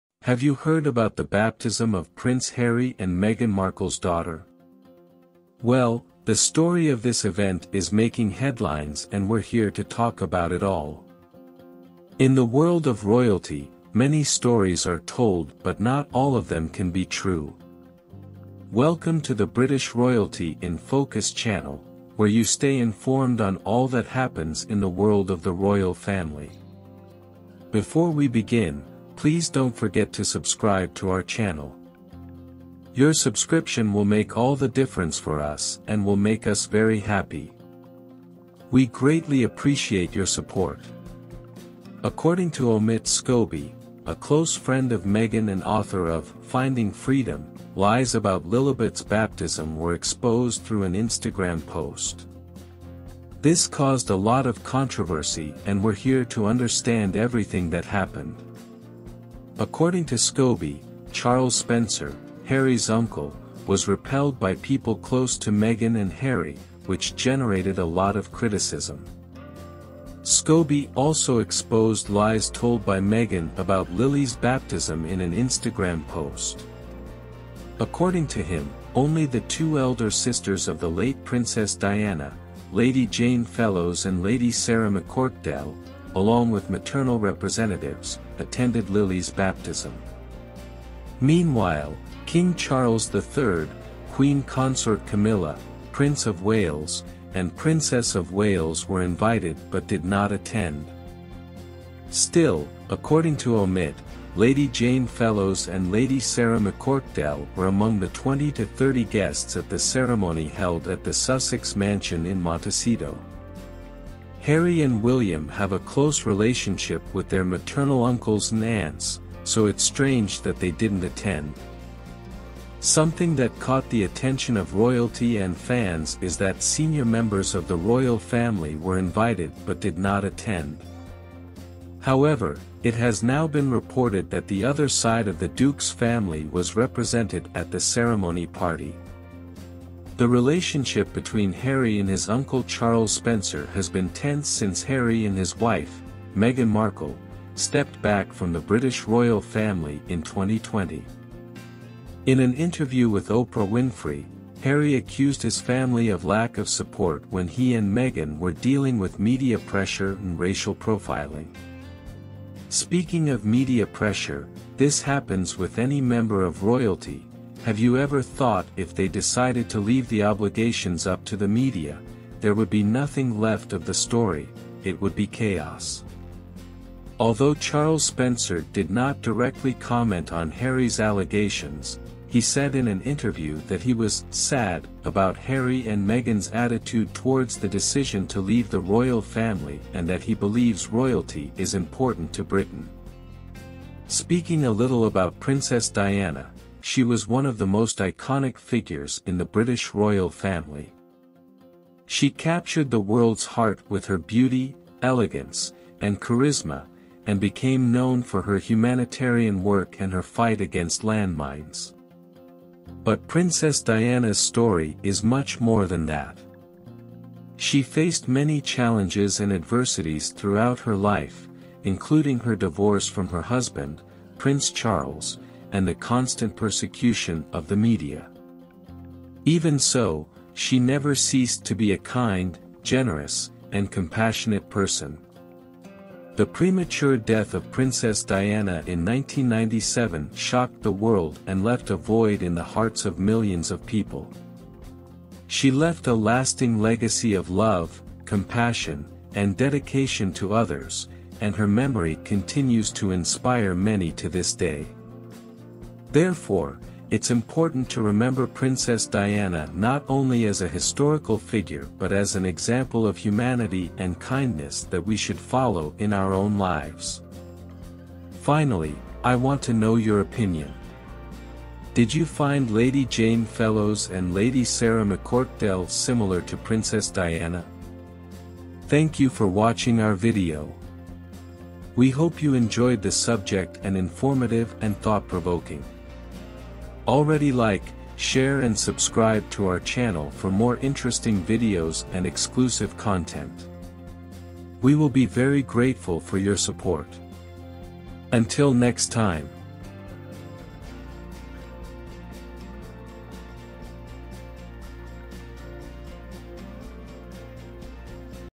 THE SPENCERS REPEL! Charles Spencer Made Meghan Furious About His Lies Regarding The Baptism Of Lil!
In the world of royalty, many stories are told, but not all of them are true. Recently, there was an uproar surrounding the christening of Prince Harry and Meghan Markle's daughter. Omit Scoby, author of Finding Freedom and close friend of Meghan, exposed the lies about Lilibet's baptism in an Instagram post. Want to know what really happened at that ceremony?

00:00 - THE SPENCERS REPEL!Charles Spencer Made Meghan Furious About His Lies Regarding The Baptism Of Lil!
00:47 - Subscribe to the channel
02:30 - Leave your like